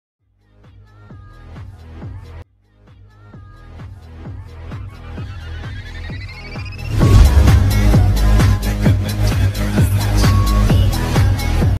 you mess with her, you mess with him ll Meme ll Not original
◇ Owlette was nearly in a fight with Owlboy though Owlette wasnt aware on who was behind her. Owlboy was scared and left leaving Owlette feeling confused till she thought she scared him off even if it wasnt her who scared him. Catboy's eyes was yellow because it was tiger eyes. Its my Au so pls understand. Thanks

☆ Characters used:
Owlboy
Owlette
Catboy

☆ Note: no part 2 and we love you ♡♡. Take care and be safe